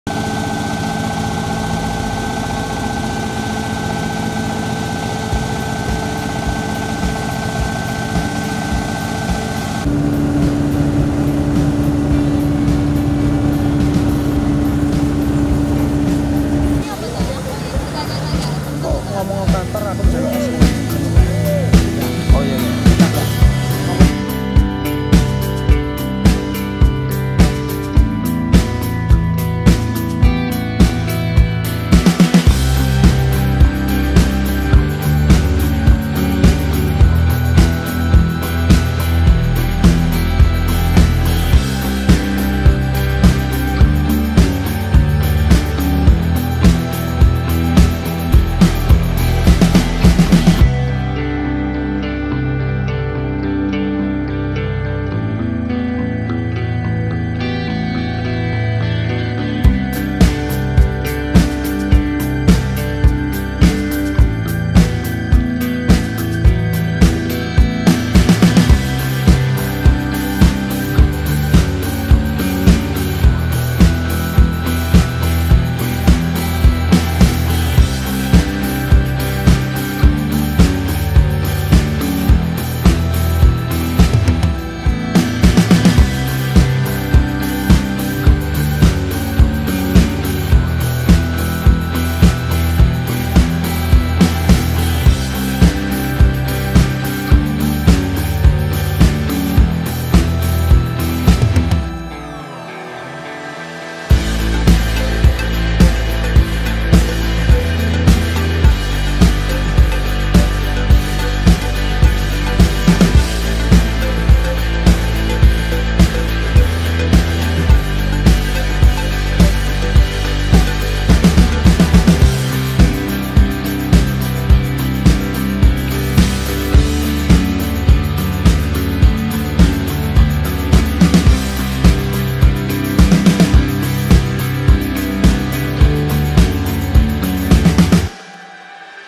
Penepung Serbaguna Kapasitas Besar | Diskmill 45 Kombi Blower | Membuat Bekatul Dari Kulit Kedelai
Jual Mesin